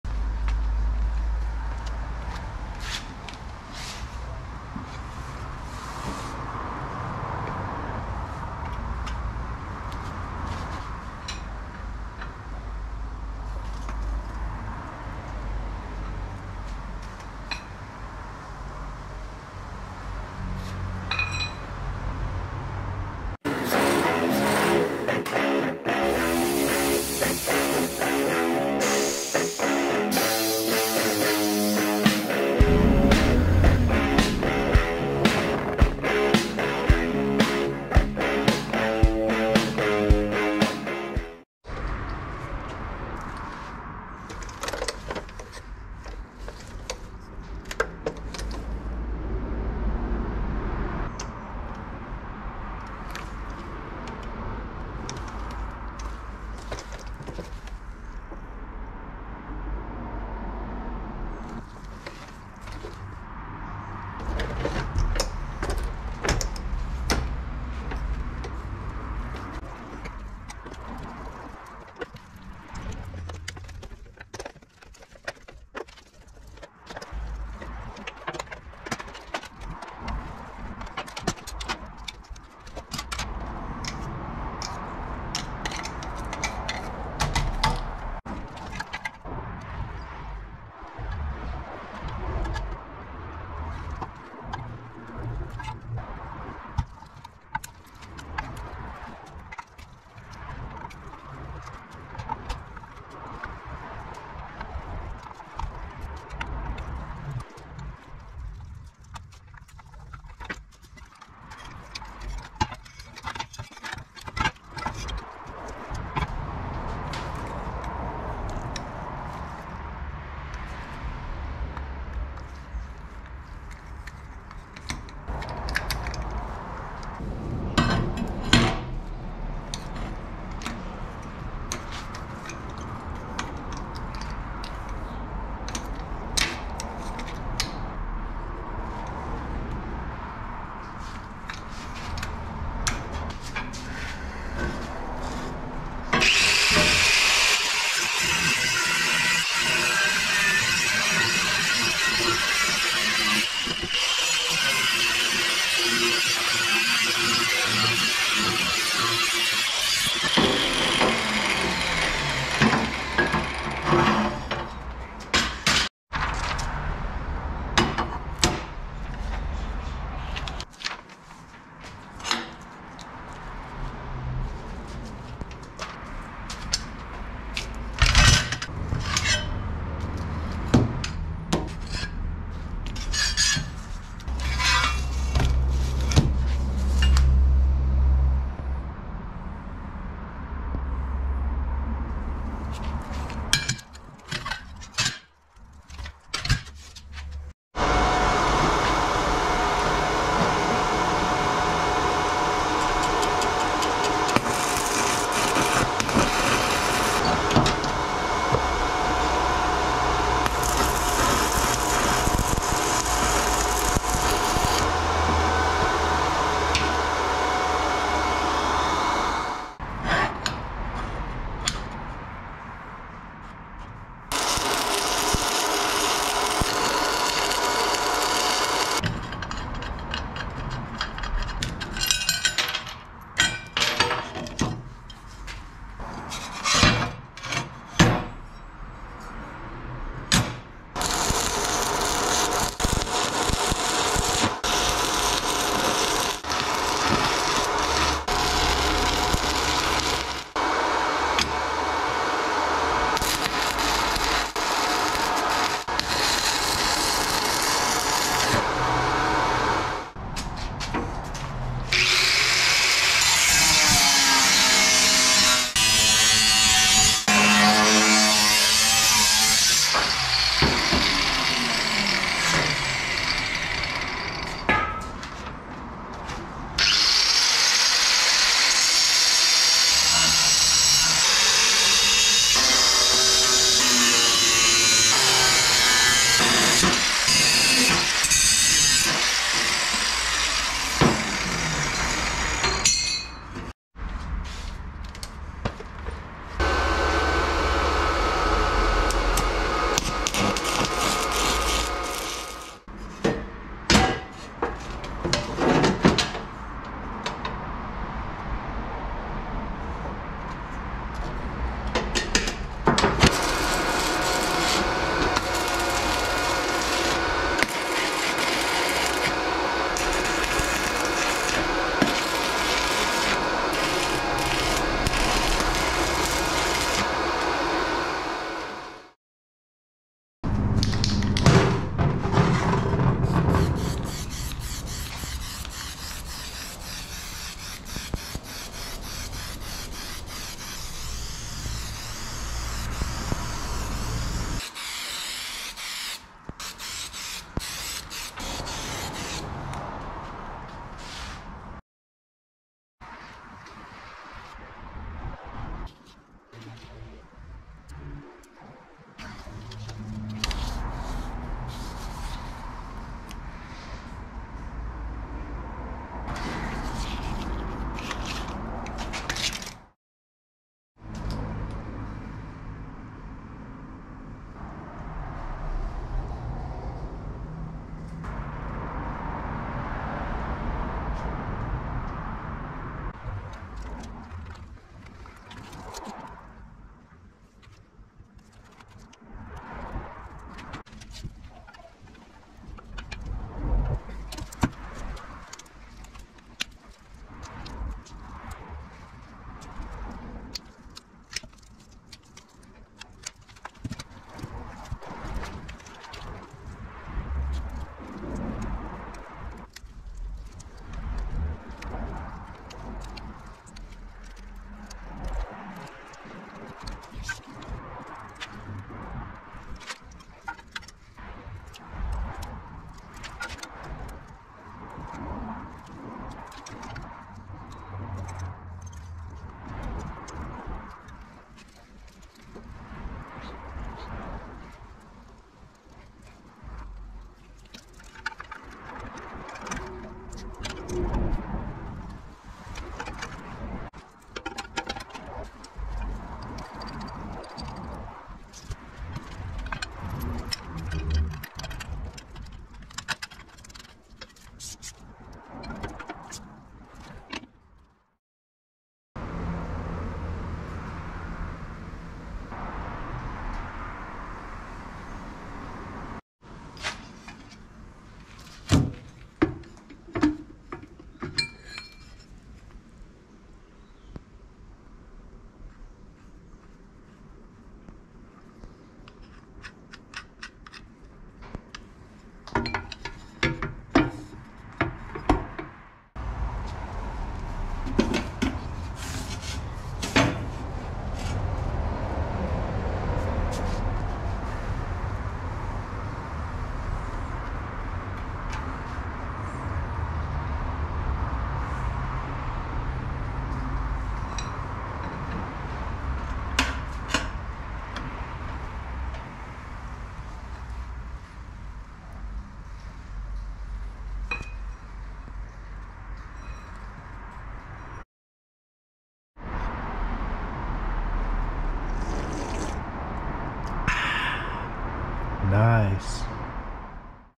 30$ DIY Scale for Weighing cars and trucks (bathroom scale hack)
How I made a scale for weighing heavy things like cars and trucks using an old bathroom scale and the power of mechanical leverage.